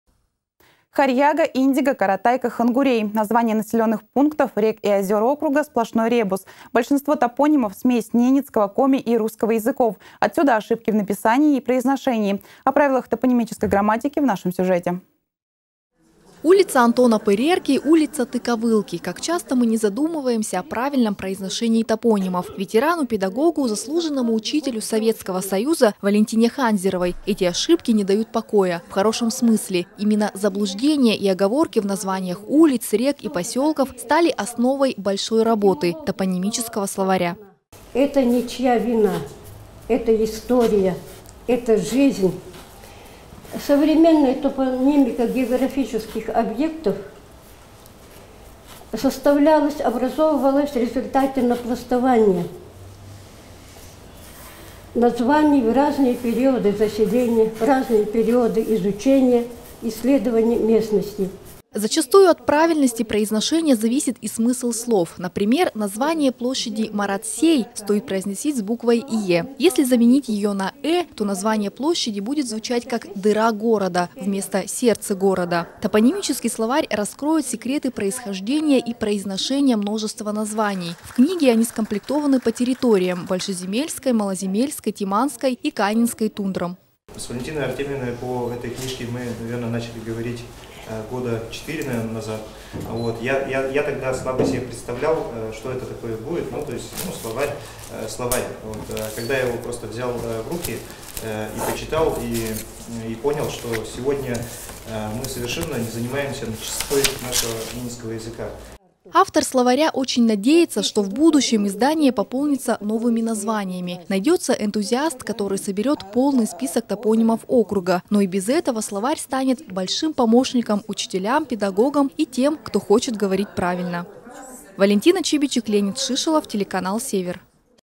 Топонимический словарь Ненецкого округа увидел свет
Харьяга, Индига, Каратайка, Хонгурей. Названия населенных пунктов, рек и озер Ненецкого округа сплошной ребус. Как часто мы не задумываемся о их правильном произношении. Ветерану-педагогу, заслуженному учителю Советского Союза Валентине Ханзеровой эти ошибки не дают покоя. Именно заблуждения и оговорки в названиях улиц, рек и поселков стали основой большой ее работы — топонимического словаря.